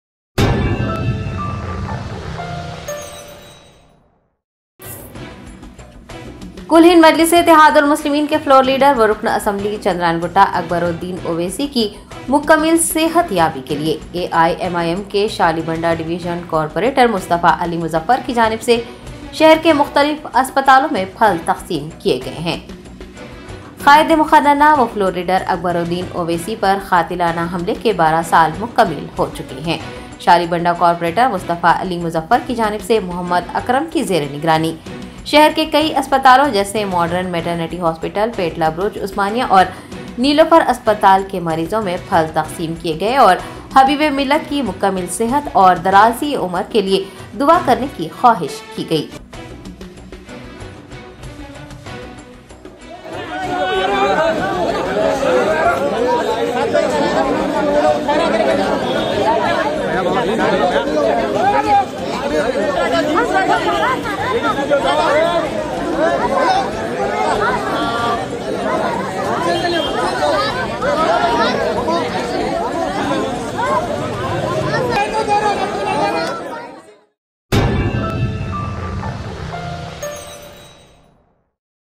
Fruits Distributed for Healthy and Long Life of Akbar Owaisi
Join hand in hand in his mission to promote healthy living by distributing fruits for a long and fulfilling life of Akbar Owaisi. Discover the benefits of incorporating fruits into your daily diet and learn how it can improve your overall health. Watch now and join the movement!